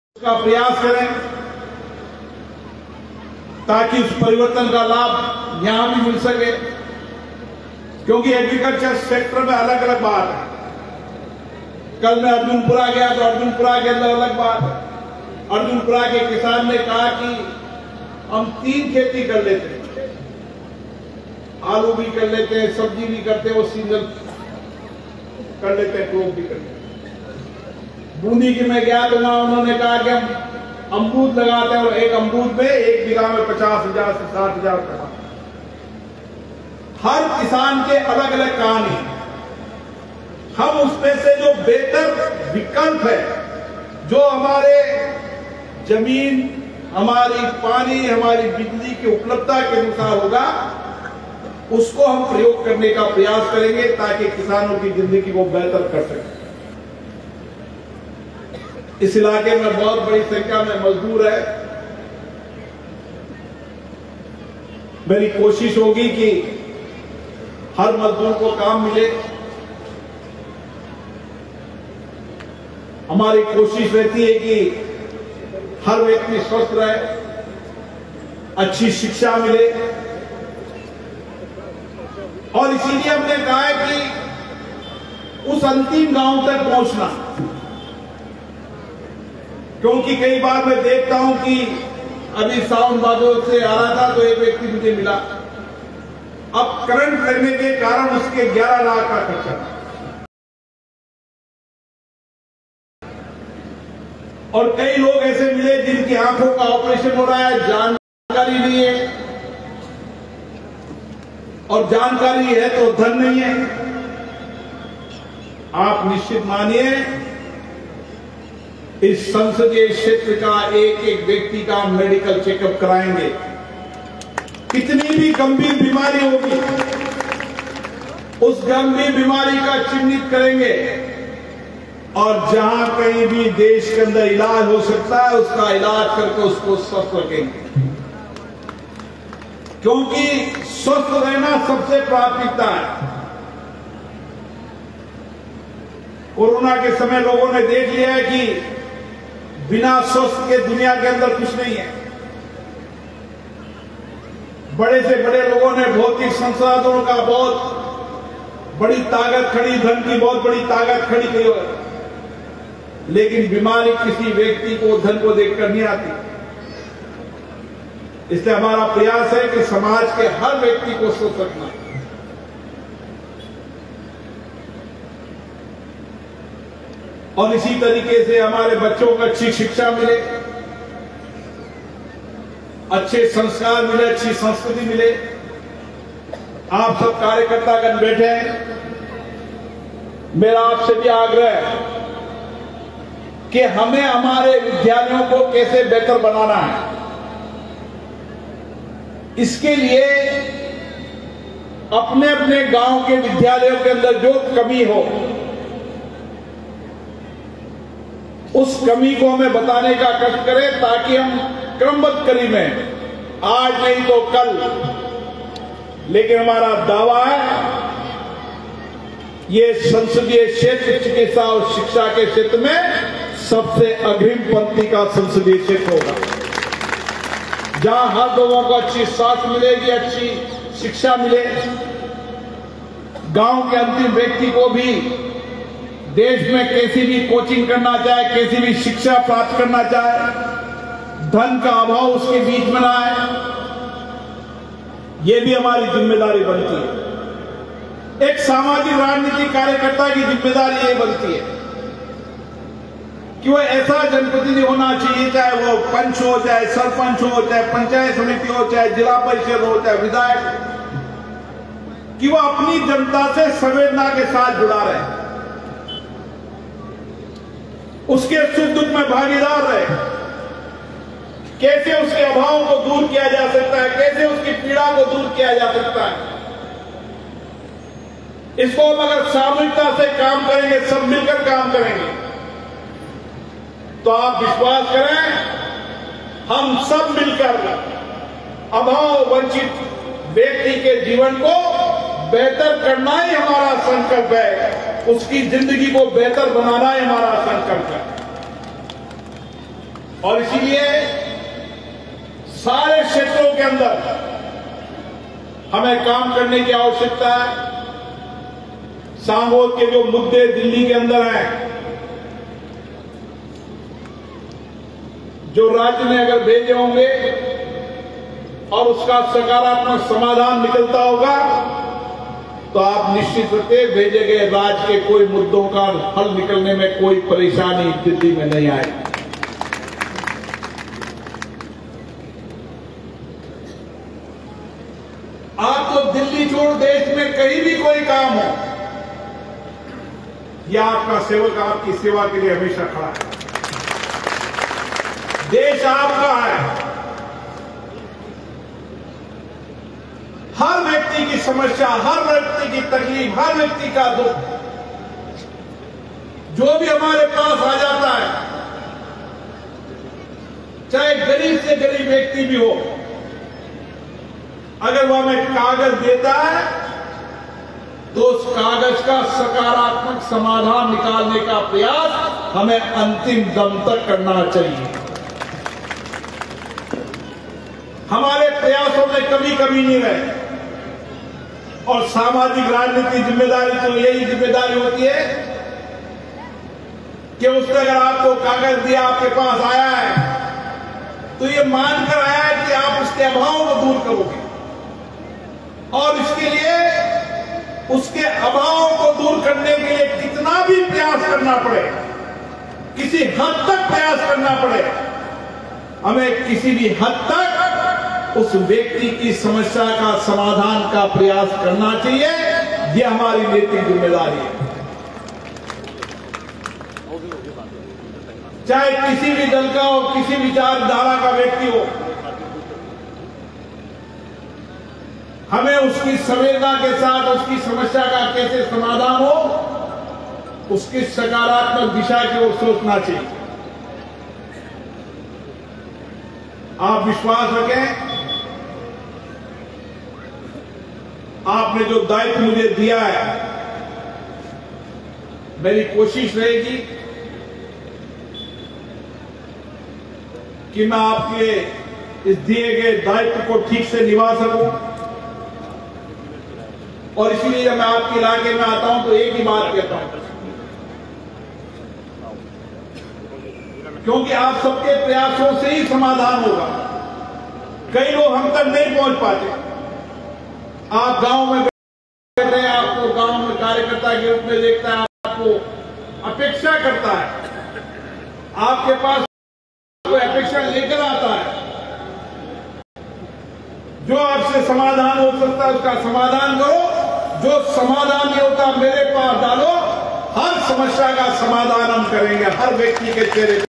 लोक सभा अध्यक्ष ओम बिरला भाषण | सांगोद विधानसभा | स्नेह मिलन कार्यक्रम | Om Birla speech in Sangod
सांगोद विधानसभा के दीपावली स्नेह मिलन कार्यक्रम में कर्णेश्वर धरा पर लोकसभा अध्यक्ष श्रीमान ओम बिरलाजी का अभूतपूर्व आत्मिक अभिनंदन स्वागत।
स्नेह मिलन कार्यक्रम

Kansas sangod kota